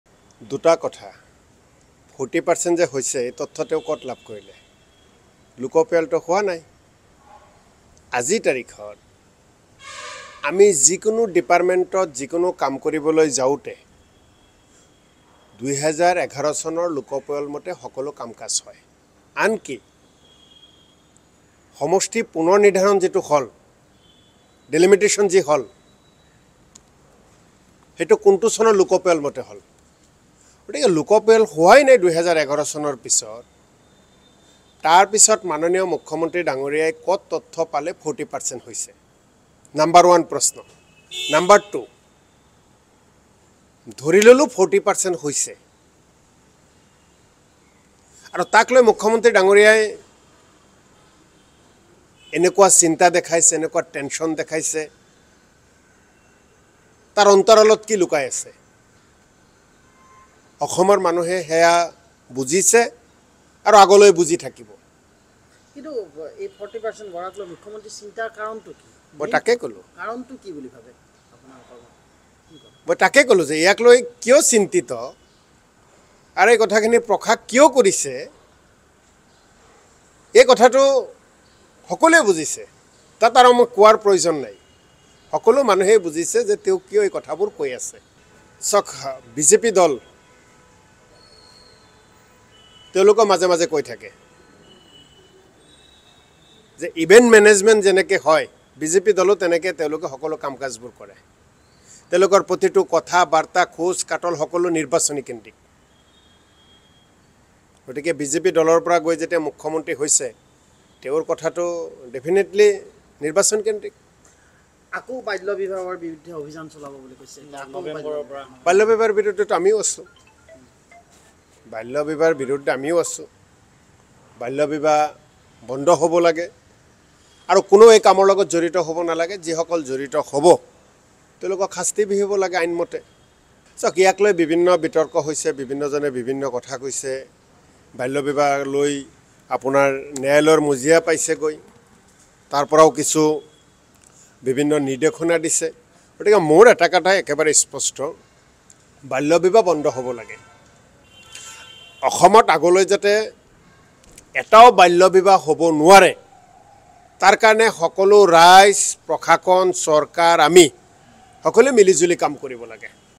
MLA Jakir Hussain | লোকপ্ৰিয়ল নোহোৱাকৈ CMয়ে 40% মুছলমানৰ তথ্য ক'ত পালে?জাকিৰ হুছেইন শিকদাৰ N18V
২০১১ ৰ পৰা হোৱা নাই লোকপ্রিয়ল | মুখ্যমন্ত্ৰীয়ে কোৱা কথাৰ অন্তৰালত কিবা লুকাই আছে | ইভেণ্ট মেনেজমেণ্টৰ নিচিনা তেওঁলোকে কথা কয় | মুখ্যমন্ত্ৰীয়ে যি কথা কলে,সেইয়া নিৰ্বাচন কেন্দ্ৰিক |

লগতে চাওঁক দিনটোৰ বহু গুৰুত্বপূৰ্ণ খবৰ |